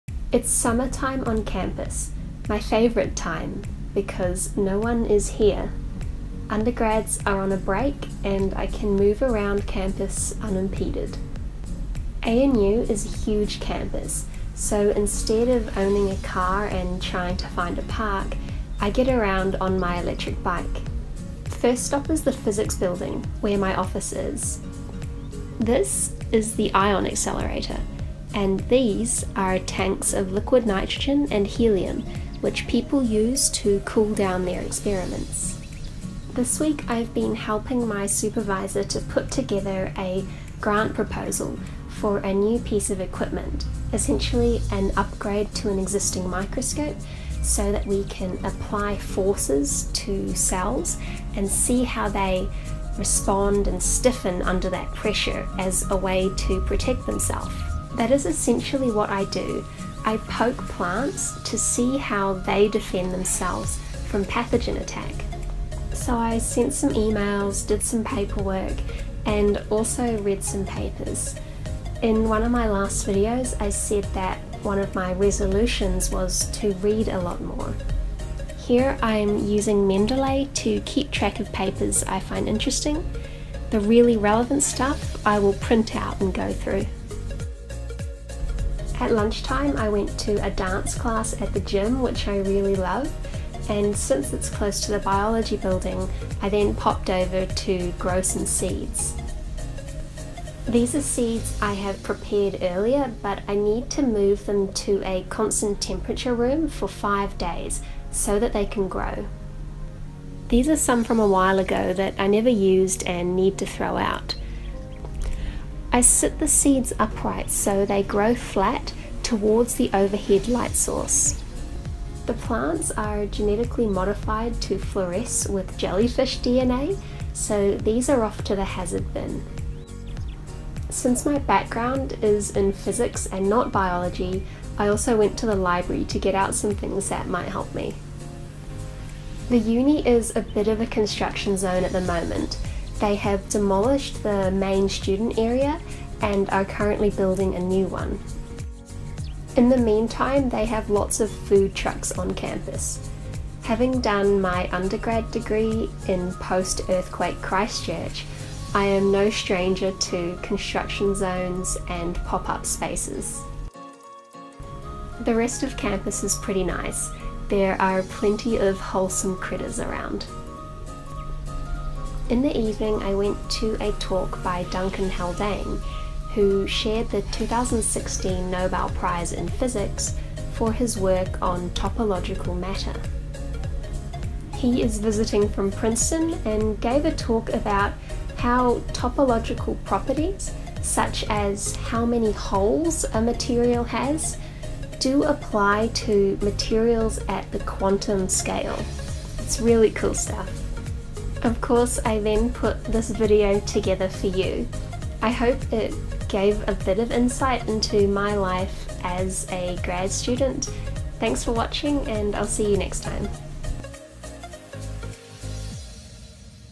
Day in My Life as a Graduate Student
I'm a physics PhD student, and this was a day in my life.